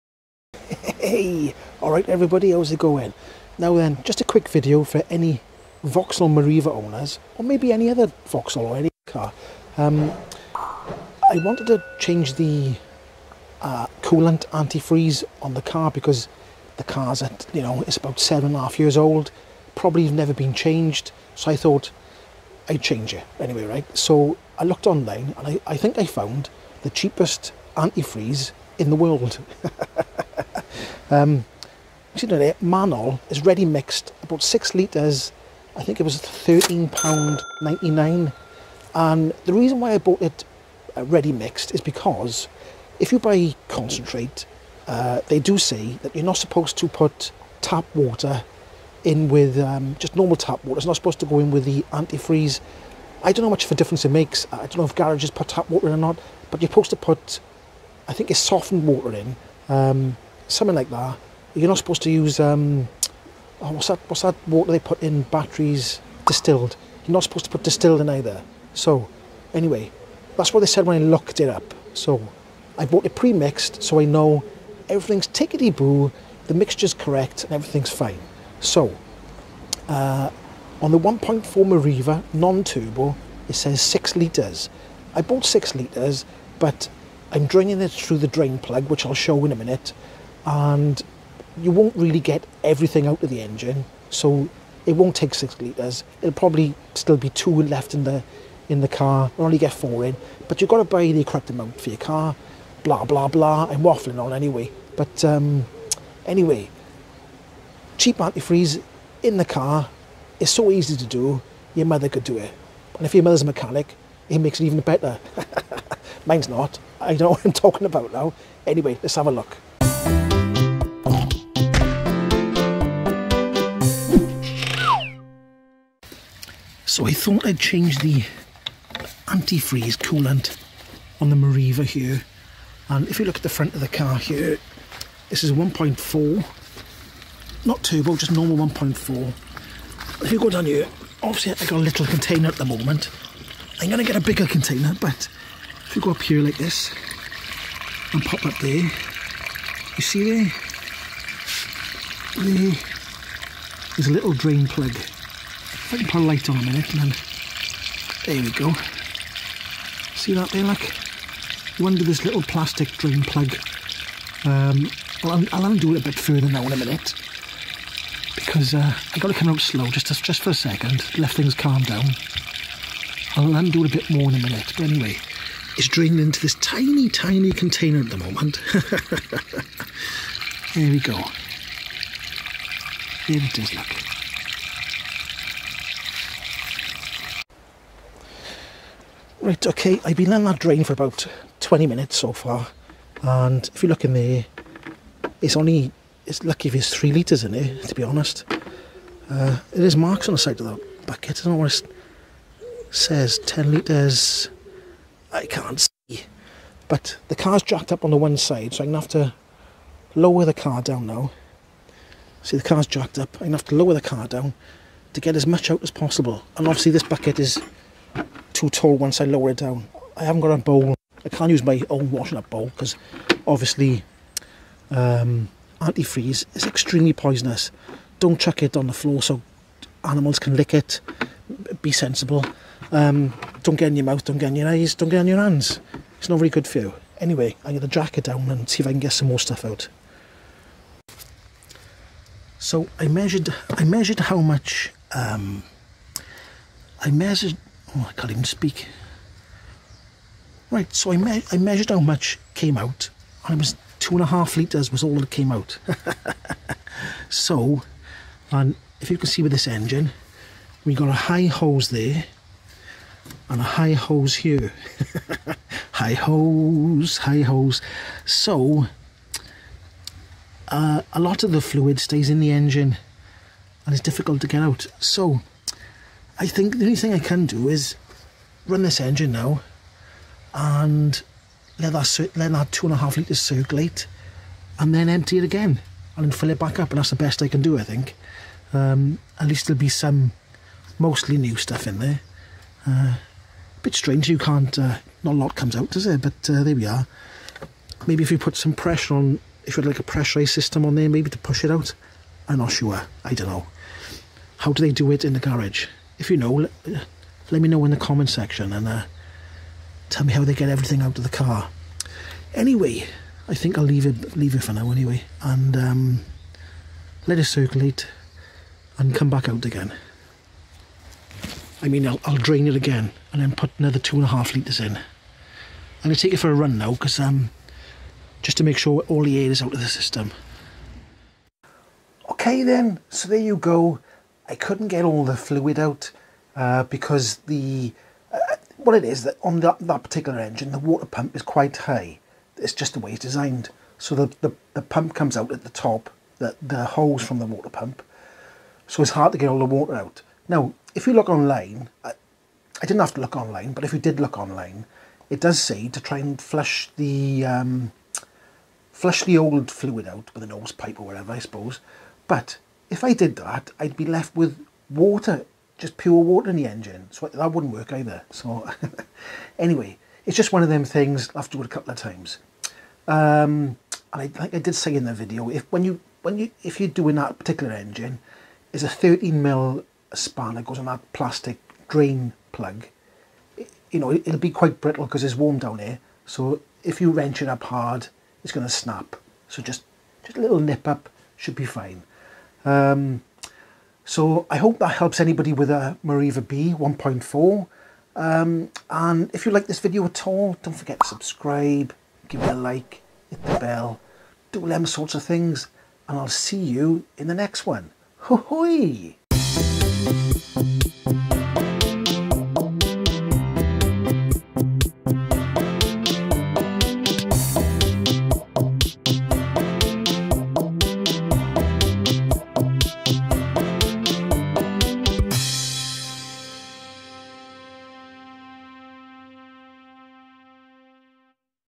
THE CHEAPEST ANTIFREEZE DELIVERED TO DO THE COOLANT CHANGE ON A 1.4 MERIVA B
AMAZON WISH LIST

COOLANT ANTIFREEZE USED
6L Coolant Antifreeze G12+ RED Ready Mixed

MANNOL Coolant G12+ Ready Mixed

4212 G12+ MANNOL

ENGINE WAS A VAUXHALL  MERIVA B 1.4 A14XER